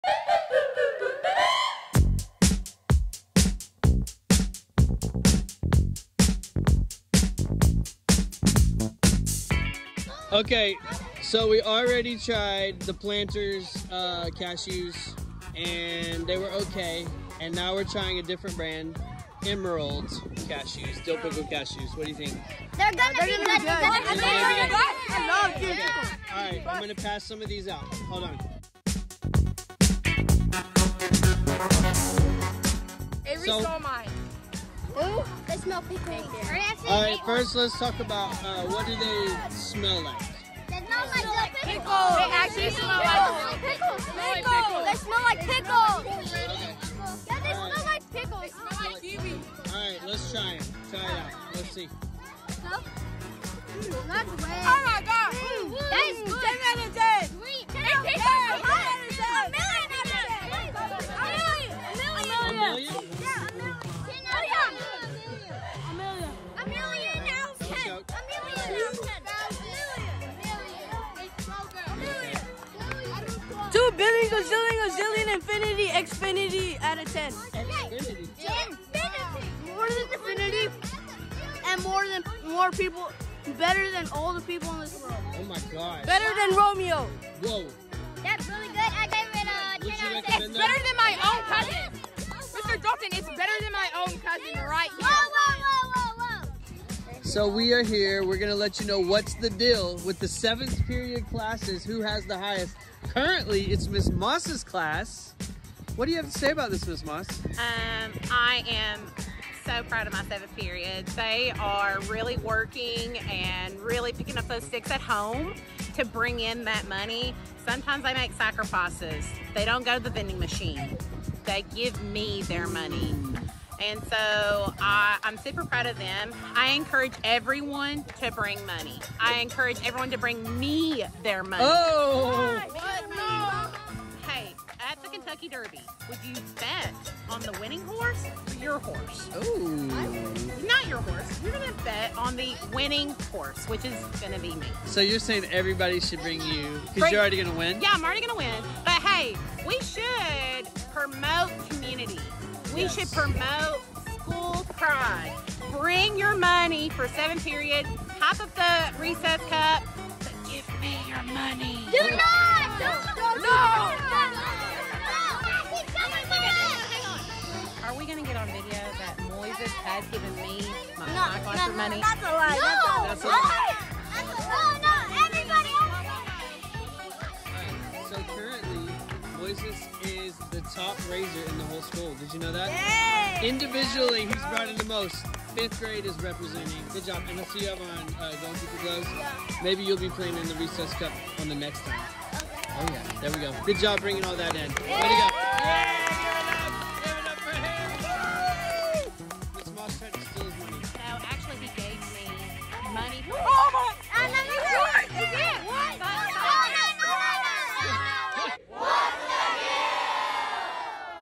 What’s the Dill? - Episode 11 - Emerald Dill Pickle Cashews
In this episode of What’s the Dill?, Pickle Clubbers try Emerald Dill Pickle Cashews. We also let you know what’s the dill with the Recess Cup Final 7th period fundraiser.